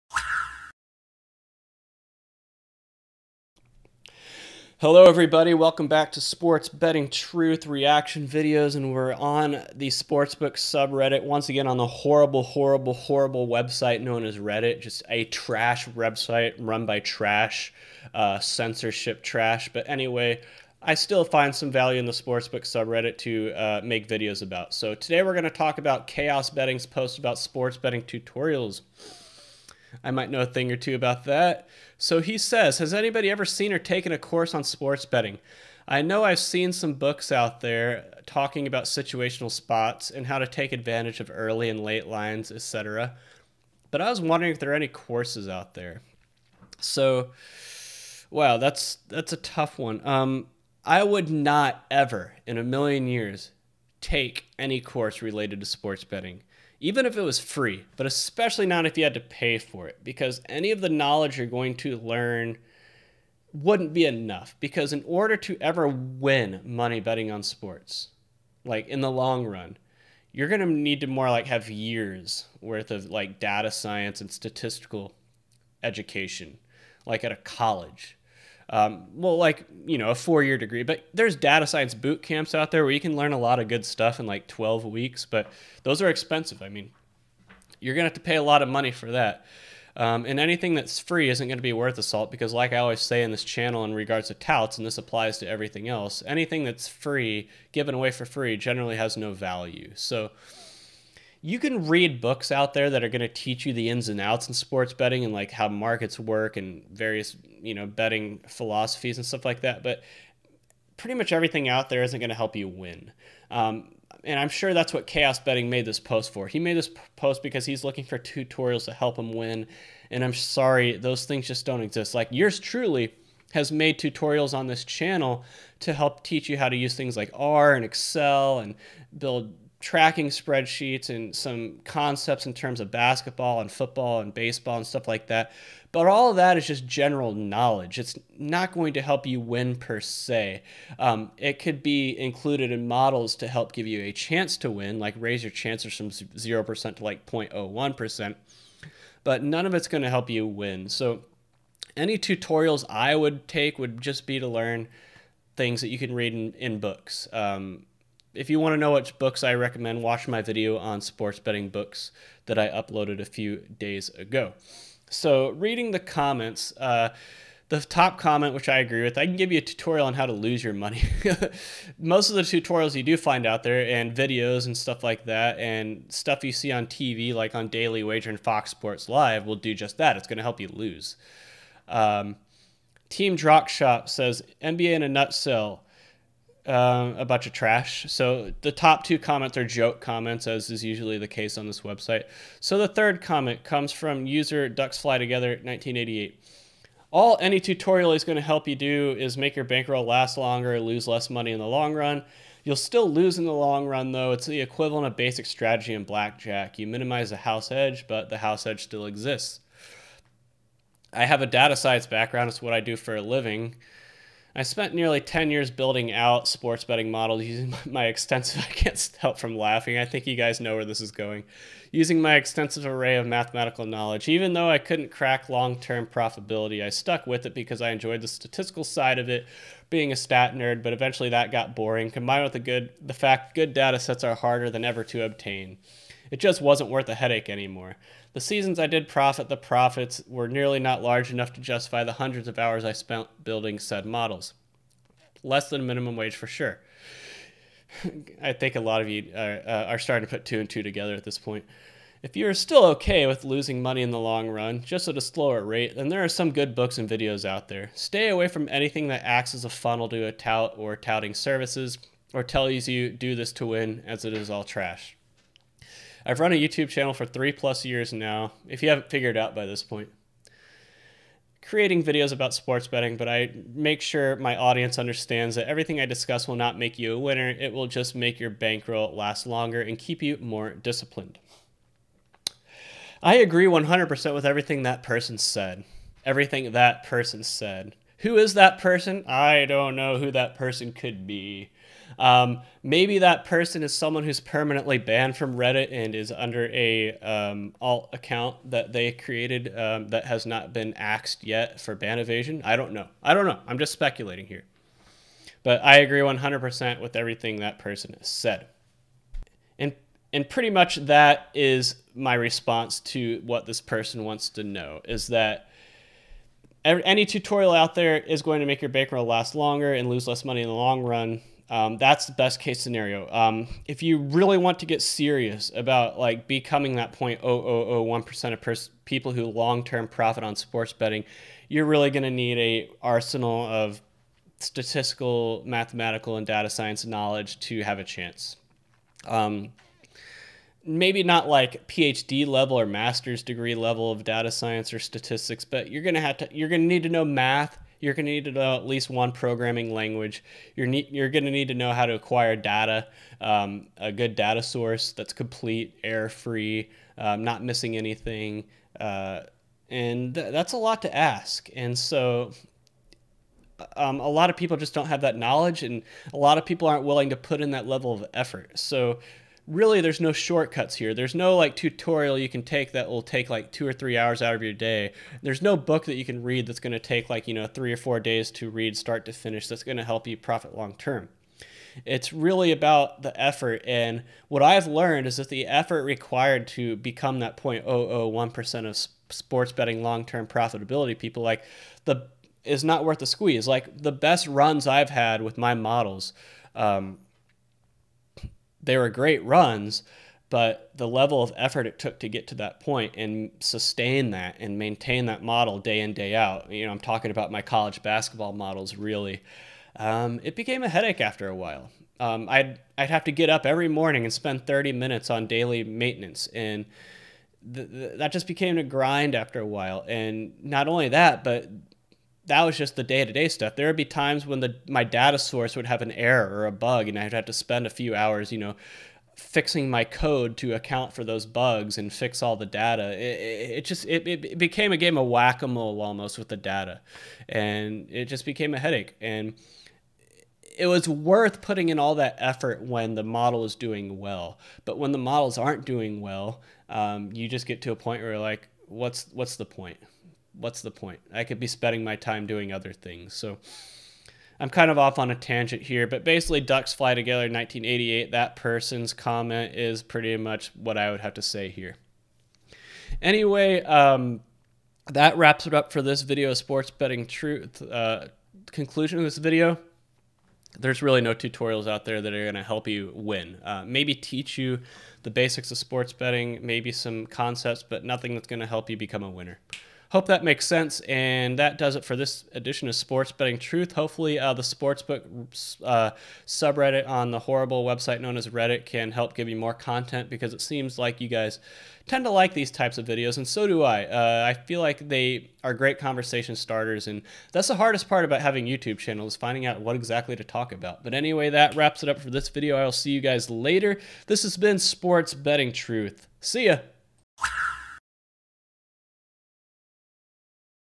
SPORTS BETTING TUTORIALS - Are There Any Out There That Can Help You Win? | Reaction Series
In this video I respond to a /r/sportsbook post on Sports Betting Tutorials and as someone who has created several of them, I give my 2 cents on what you can realistically expect to get out of these and whether or not paying for any of them is worth it.